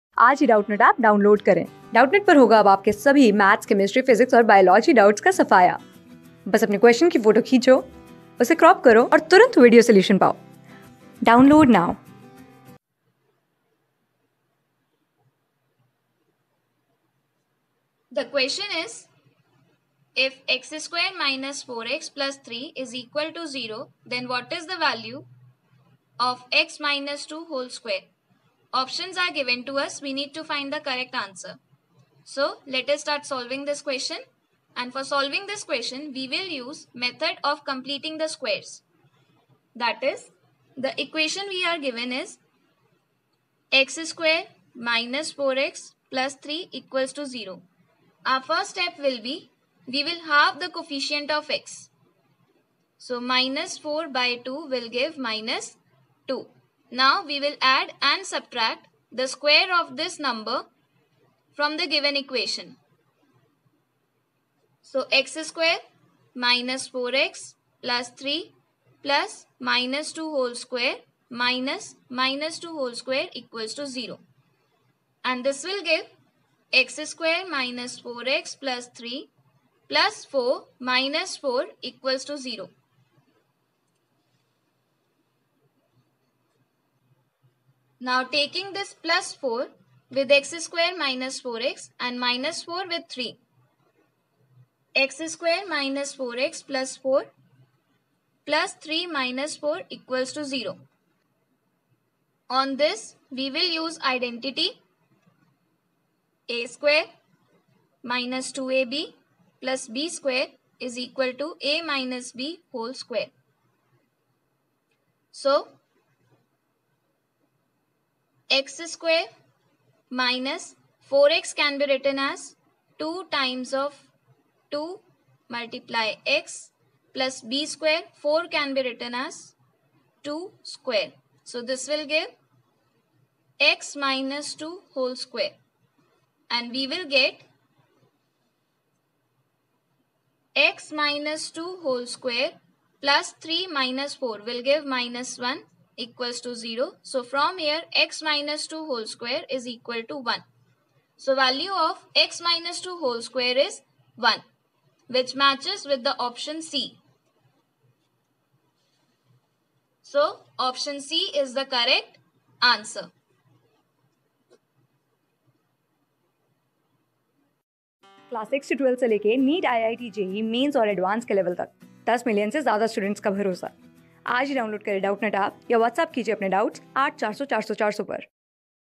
If x^2 - 4x + 3 = 0, then what is the value of (x - 2)^2? | CLASS 12 | FACTORING | MATHS | Doub...
If x^2 - 4x + 3 = 0, then what is the value of (x - 2)^2?
Class: 12
Subject: MATHS
Chapter: FACTORING
Board:SAT

👉 Have Any Query? Ask Us.